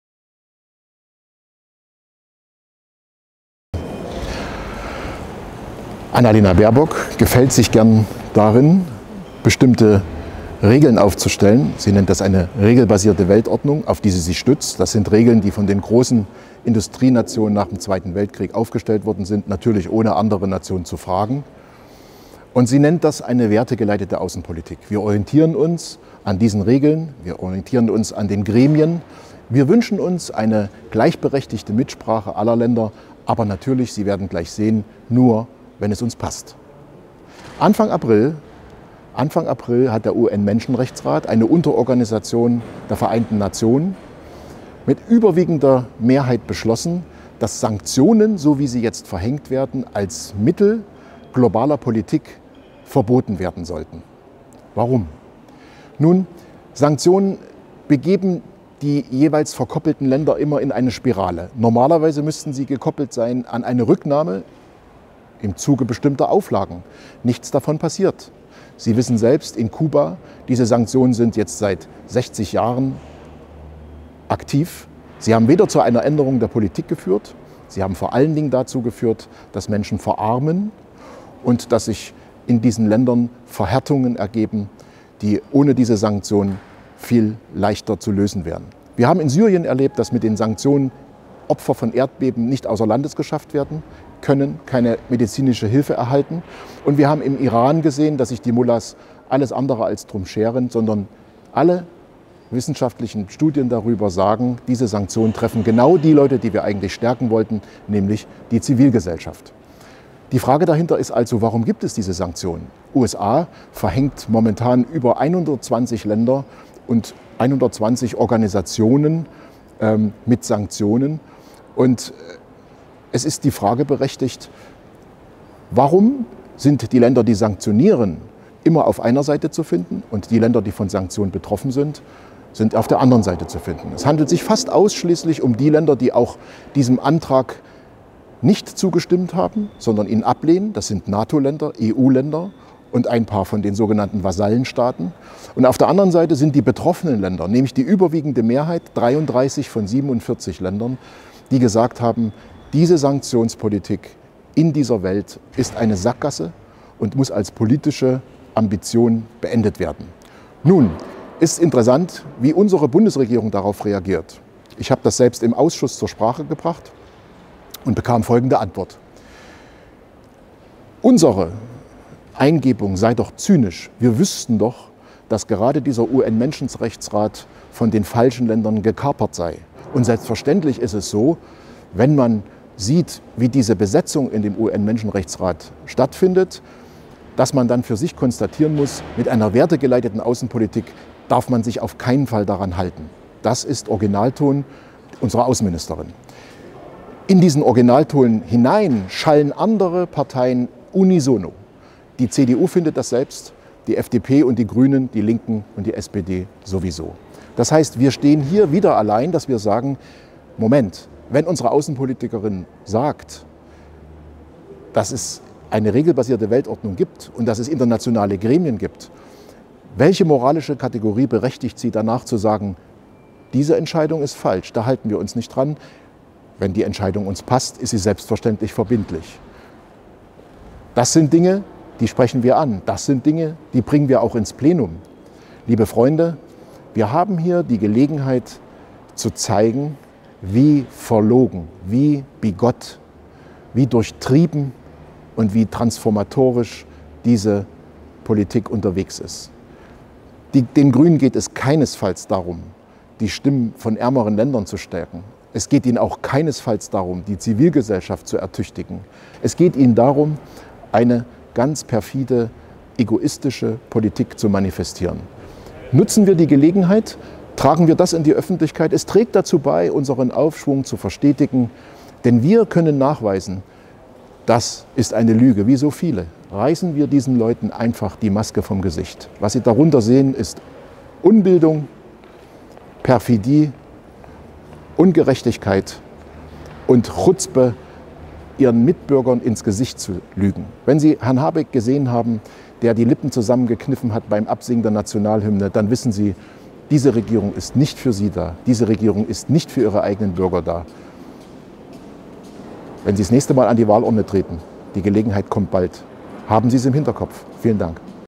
Sanktionen als Werkzeug imperialer westlicher Politik abschaffen
Der UN-Menschenrechtsrat fordert und Barbock weigert sich. Sie nennt das Werteordnung. Warum, erfahren Sie in diesem Video.